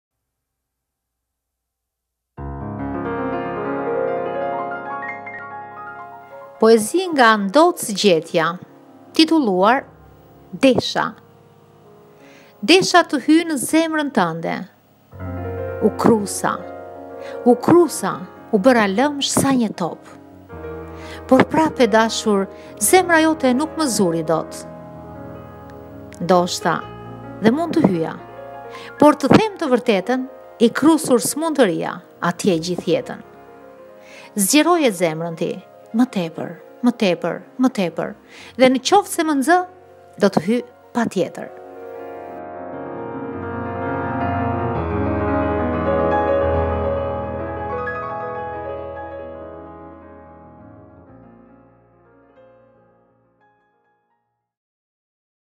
NDOC GJETJA Poezi
"DESHA"

Desha të hyj në zemrën tënde
U kërrusa,u kërrusa, u bëra lëmsh sa një top,
por prap, e dashur,
Zemra jote nuk më zuri dot.

Ndoshta dhe mund të hyja,por të them të vërtetën,
i kërrusur s'mund të rrija, atje gjithë jetën.

Zgjeroje zemrën ti! Më tepër! Më tepër! Më tepër!
dhe, në qoftë se më nxe,
do të hyj pa tjetër...